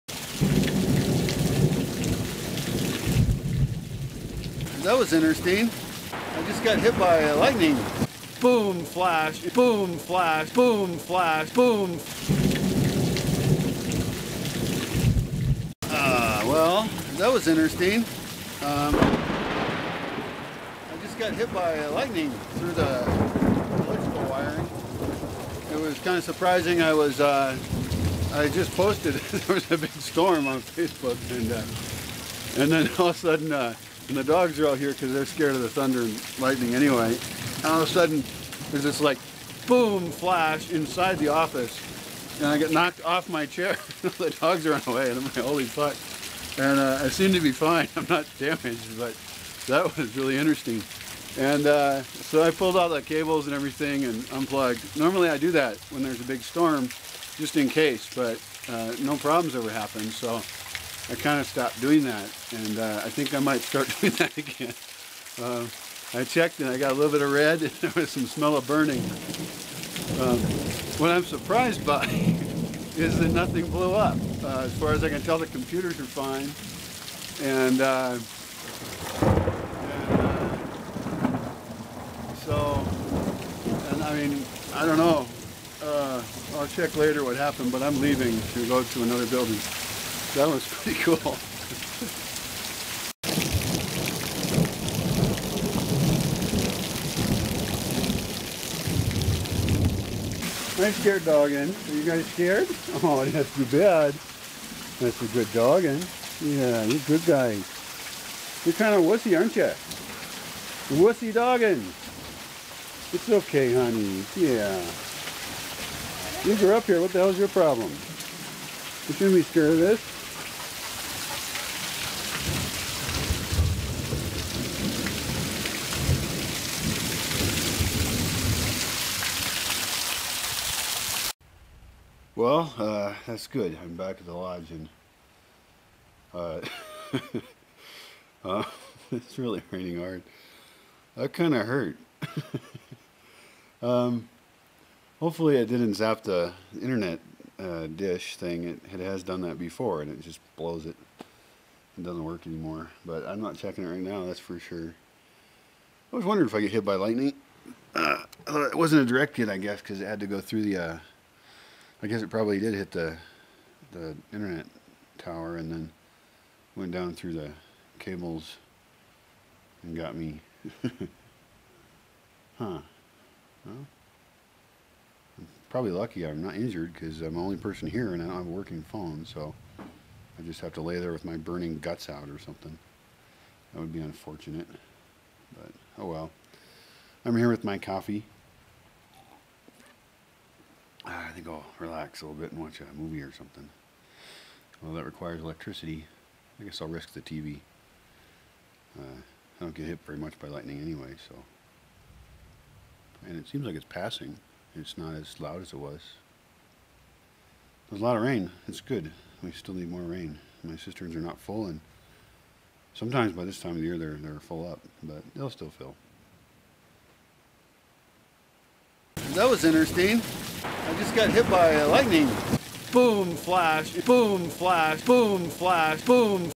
Lightning Strikes in the Bosque.
It wasn't really a direct hit. It hit the internet tower, then came down the electrical cable and there was a big flash and I was knocked off my chair. Mostly undamaged.

What was it like?